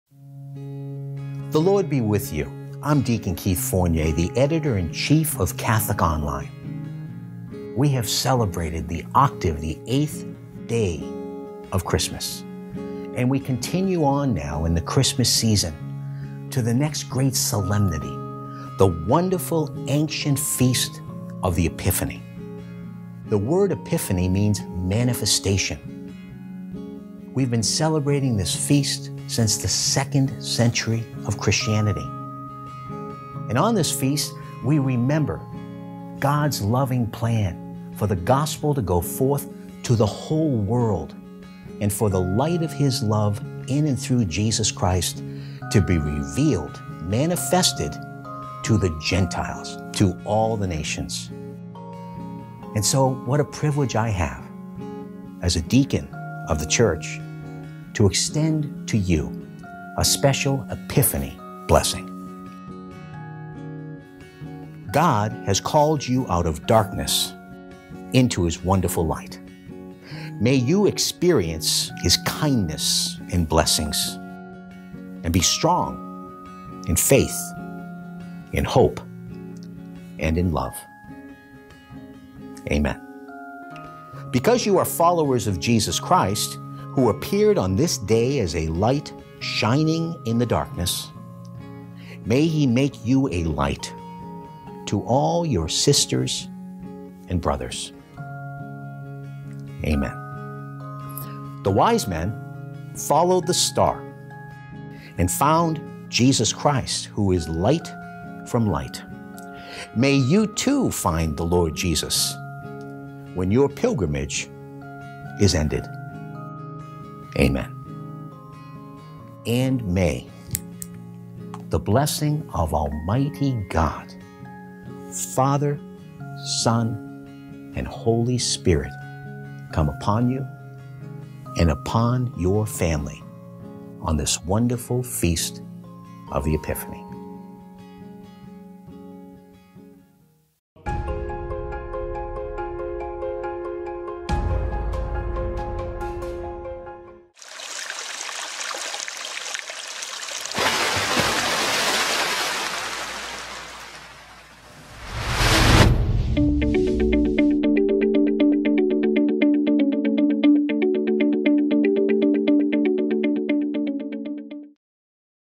Epiphany Blessing HD
The Lord be with you. I'm Deacon Keith Fournier, the editor-in-chief of Catholic Online.

We have celebrated the octave, the eighth day of Christmas, and we continue on now in the Christmas season to the next great solemnity, the wonderful ancient feast of the Epiphany. The word Epiphany means manifestation.

We've been celebrating this feast since the second century of Christianity, and on this feast we remember God's loving plan for the Gospel to go forth to the whole world and for the light of His Love in and through Jesus Christ to be revealed, manifested to the Gentiles, to all the nations.

And so, what a privilege I have, as a Deacon of the Church, to extend to you a special Epiphany Blessing.

God has called you out of darkness, into his wonderful light. May you experience his kindness and blessings. And be strong, in faith, in hope, and in love. Amen.

Because you are followers of Jesus Christ, who appeared on this day as a light shining in the darkness, may he make you a light- to all your sisters and brothers. Amen.

The wise men followed the star, and found Jesus Christ, who is Light from light. May you, too, find the Lord Jesus when your pilgrimage is ended. Amem.

And may the blessing of Almighty God, Father, Son and Holy Spirit come upon him, and upon your family on this wonderful feast of the Epiphany.